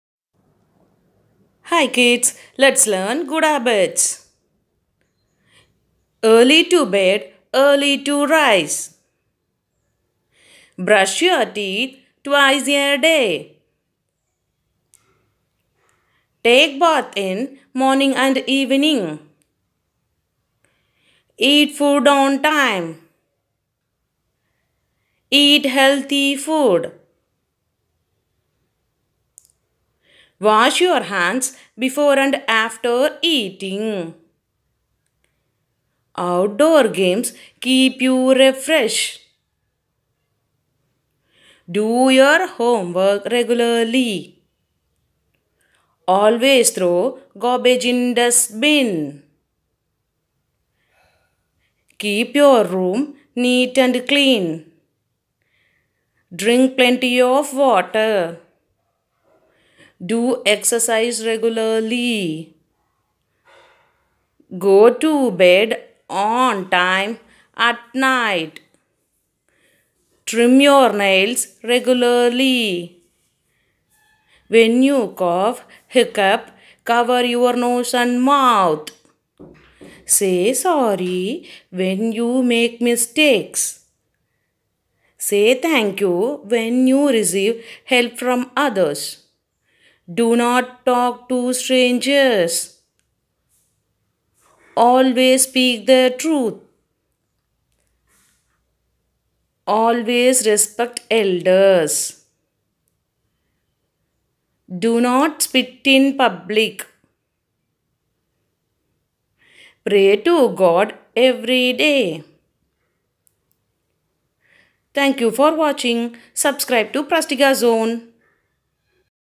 Good Habits,Good habits for kids
Good habits, good habits for kids,good habits for kids in English, good manners
Hi friends
  This video clearly describe about the good habits to be followed by the students or kids.this video will be very helpful for the children's to grew up perfectly with good manners. Good habits can be learnt and followed  on seeingt this video